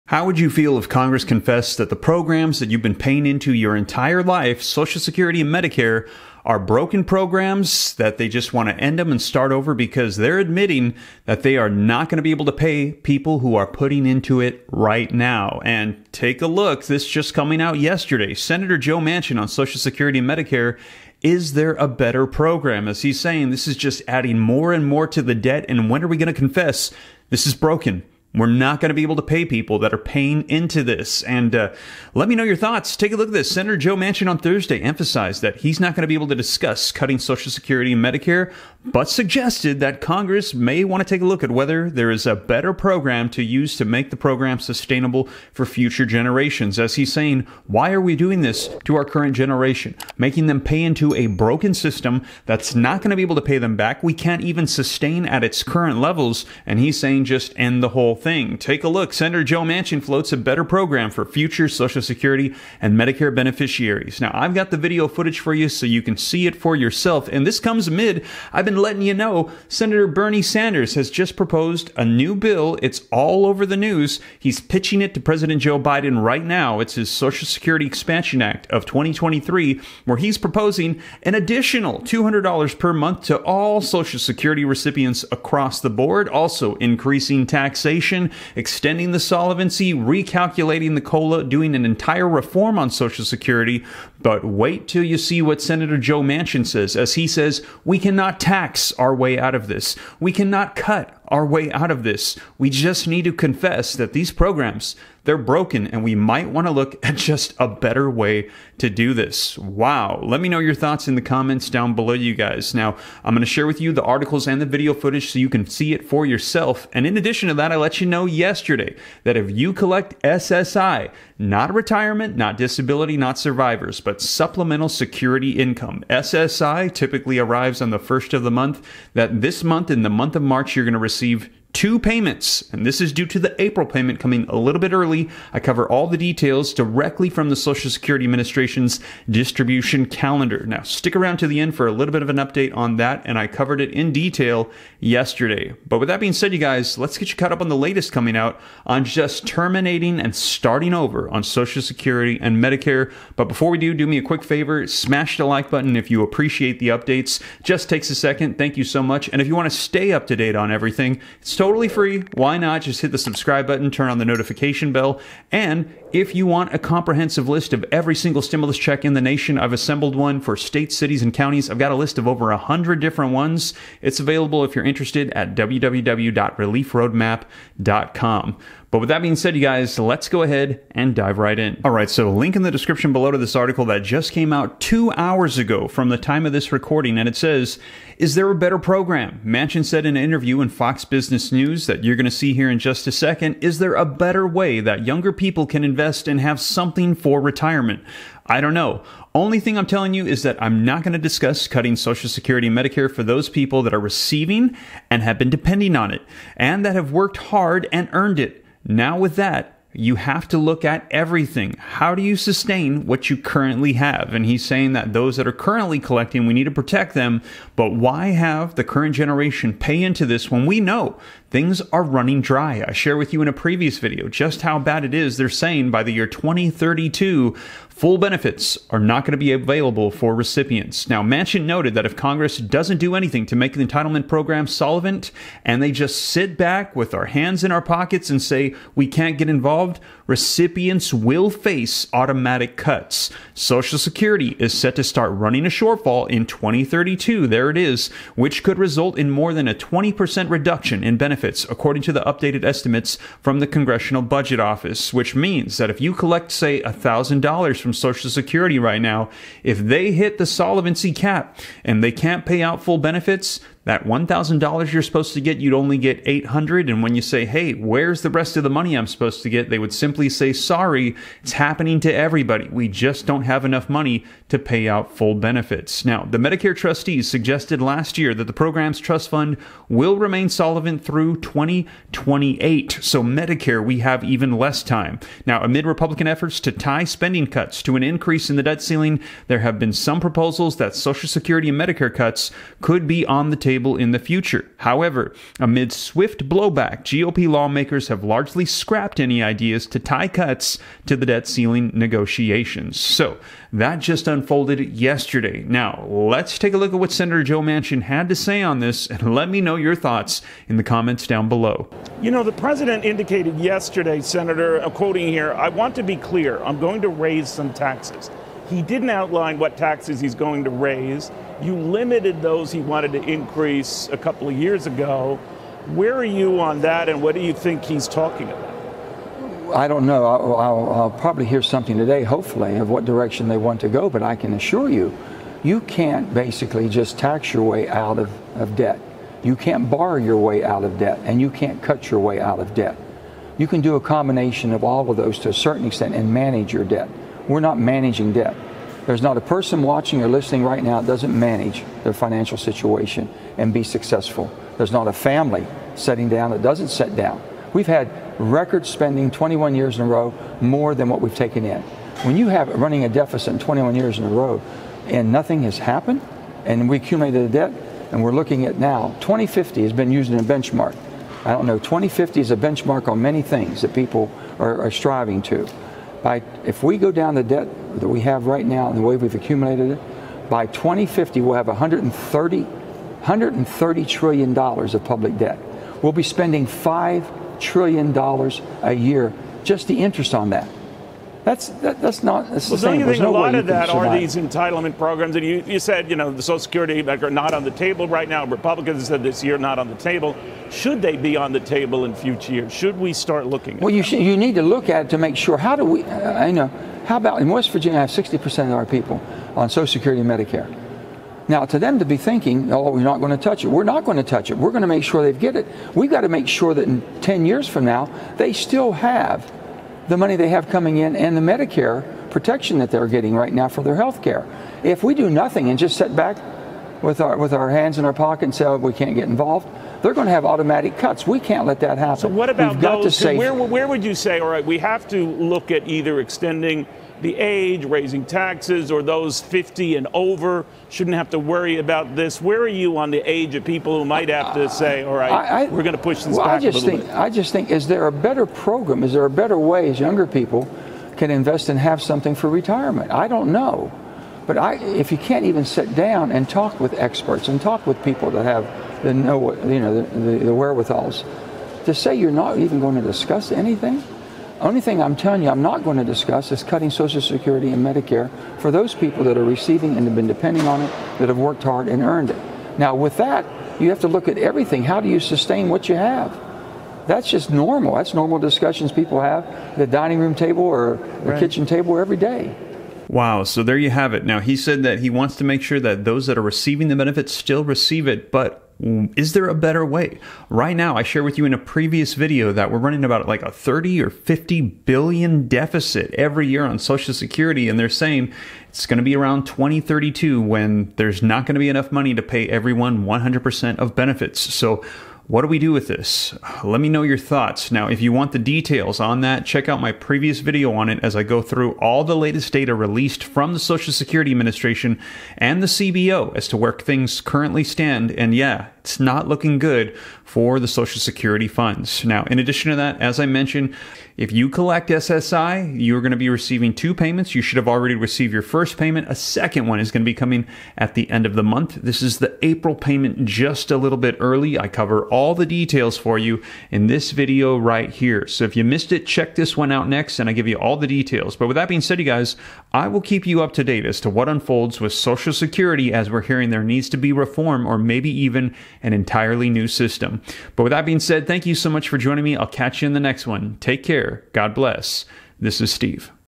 JUST IN: TERMINATE Social Security & Medicare… JUST START OVER!
Social Security & Medicare Update: “is there a better way?”
Senator Joe Manchin on Thursday emphasized that he’s not going to discuss cutting social security and Medicare but suggested that congress may have to loo at whether there is a “better program” to use to make the programs sustainable for future generations.

✦Discover over 100 stimulus checks available now around the nation (More updates arriving)

As updates come out, I will be sure to share.